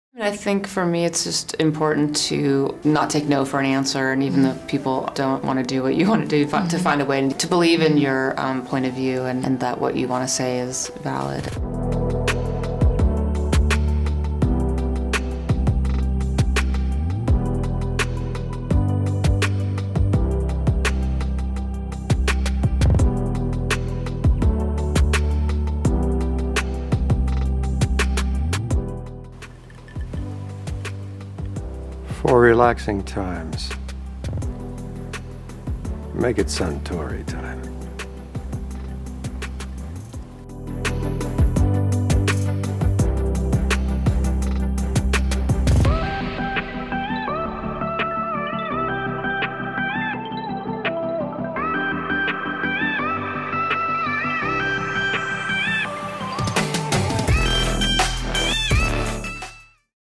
Female Filmmaker Friday: Sofia Coppola
Today for FemaleFilmakerFriday we take a look at the career of Sofia Coppola, a distinguished visionary who became the first American woman to be nominated for the Academy Award for Best Director.